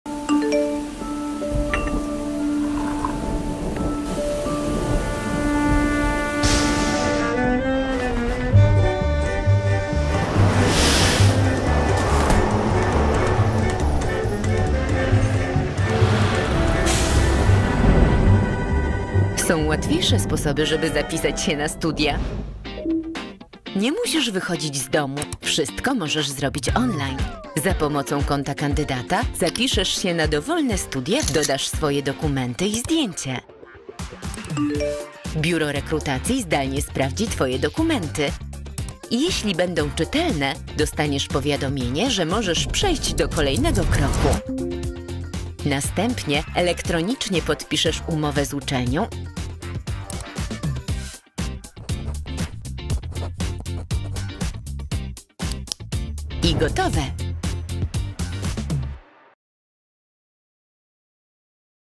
Wybierz studia i zapisz się przez internet
Nie musisz wychodzić z domu, by zapisać się na wybrane studia. Wszystko możesz zrobić online. Dowiedz się jak to zrobić!